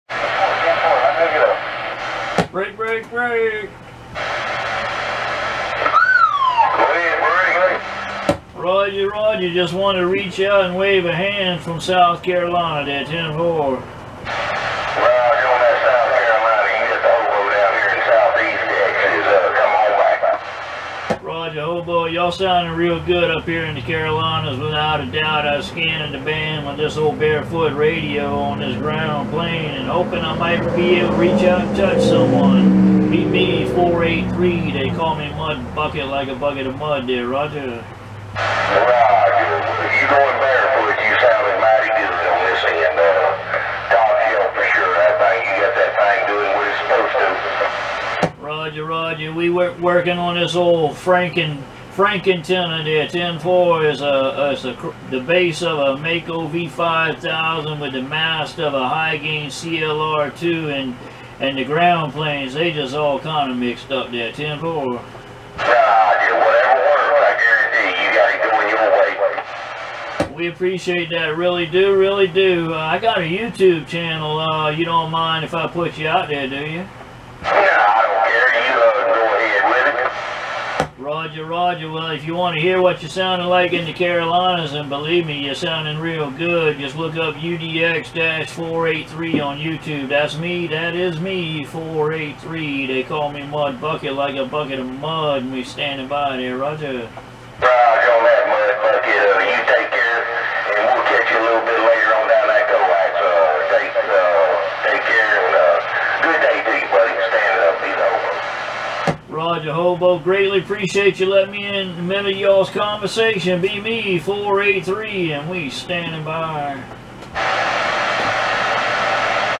HOBO South East Texas 27.615 5/29/23
I was searching high and low for a crack I could slip through to make a contact and I finally found it just a bit outside the 40. Thanks, HOBO, for letting me in there. 73